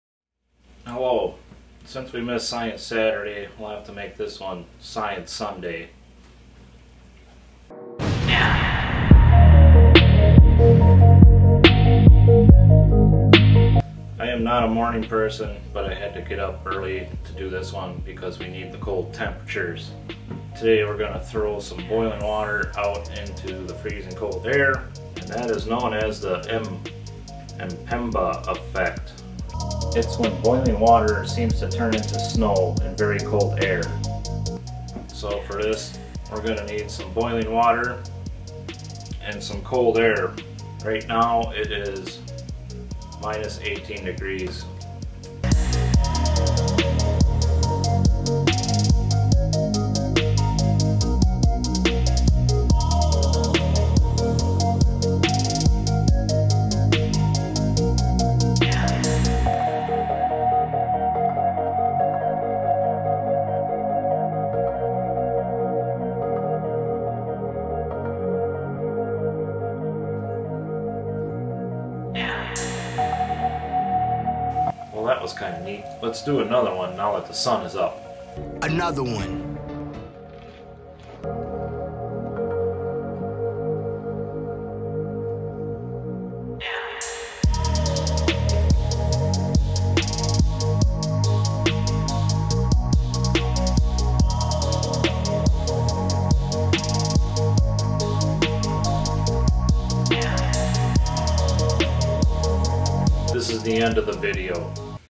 Science Sunday. The Mpemba effect. Throwing boiling water into cold air.
Throwing hot water into -18F cold air.